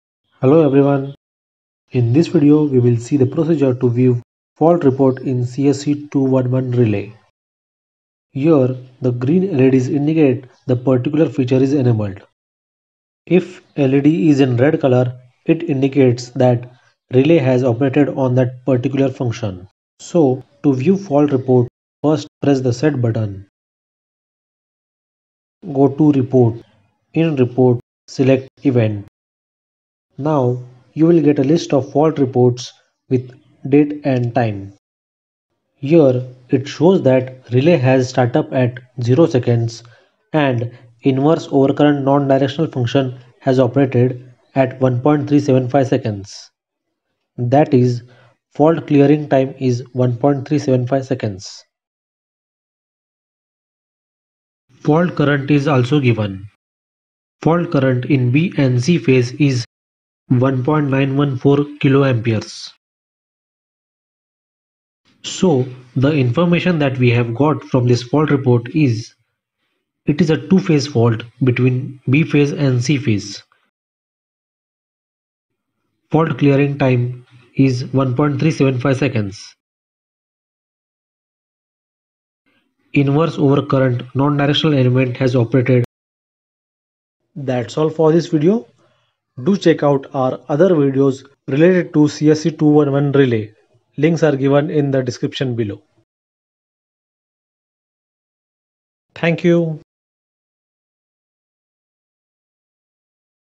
Check fault Report in CSC 211 Relay | Overcurrent & earth fault relay
CSC-211 Relay is basically a overcurrent and earth fault relay. It also has other functions such as LBB (CBF), overload etc. In this video, procedure to check fault report in CSC-211 relay is given.